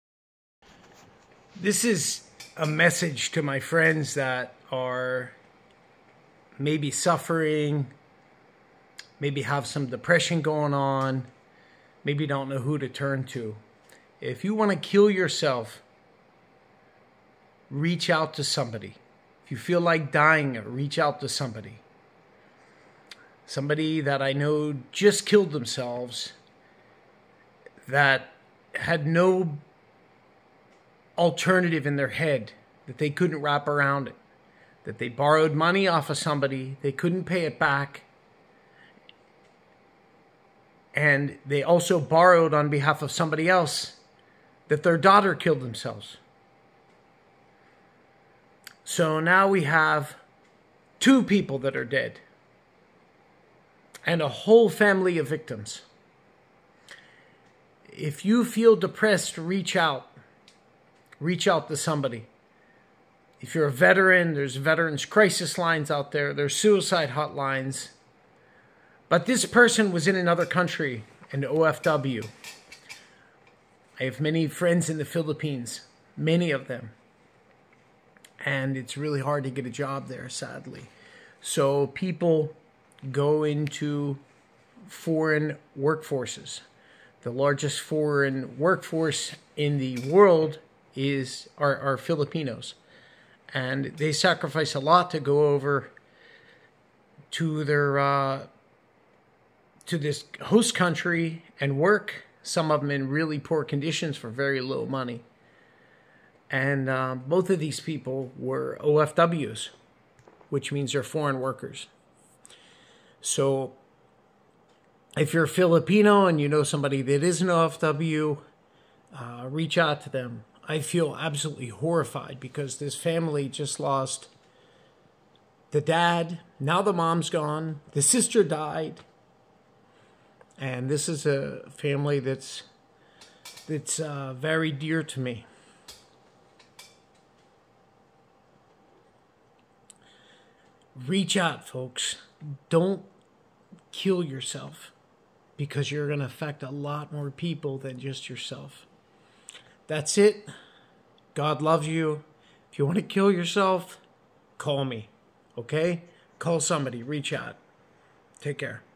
Please reach out if you are suicidal so others can help you. Philippine OFW sacrifices are real....
Prayers for my friends in Moalboal, Cebu-this is heartbreaking.  This is YEARS of working overseas nonstop, she was even denied the opportunity to return home when her own daughter died, then when her husband just died, not enough $ to return home.  She had JUST borrowed money to help another suicide victim's daughter who was an OFW too.  Such a sad story of economic enslavement that drive a person over the edge to suicide.